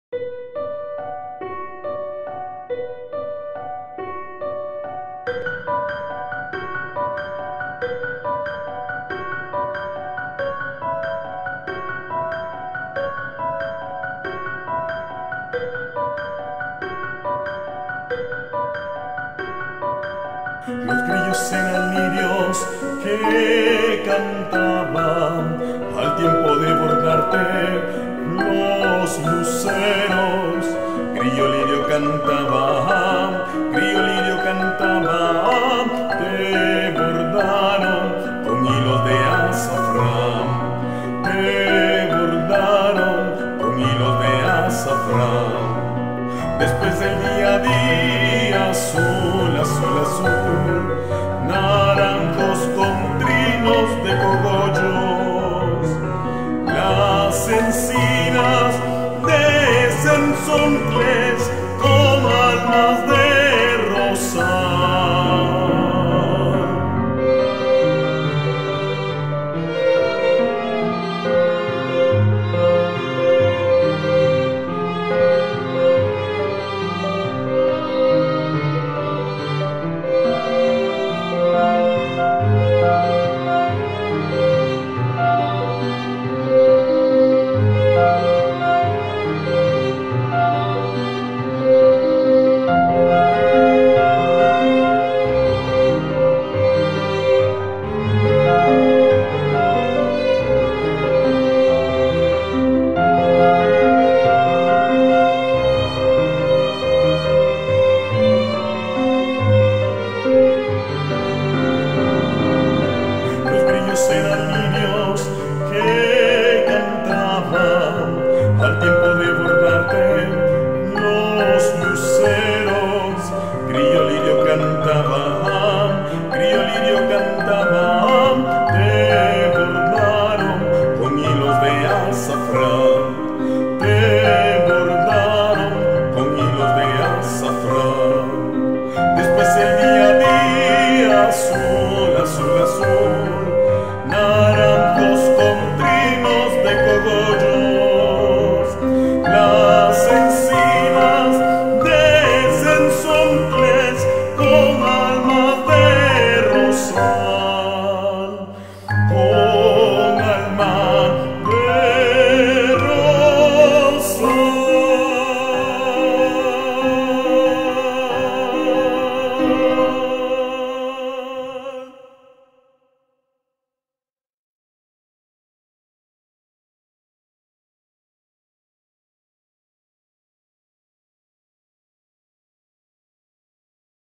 Poema Asturiano CMM 2017
Grillo Lirio, fragmento del poema de Miguel Ángel Asturias, mañana artística Colegio Monte María 2017. Arreglo y composición musical, José Fernando Aguilar Ferrer, Maestro de Música de Párvulos y Primaria
Música para la presentación de primer grado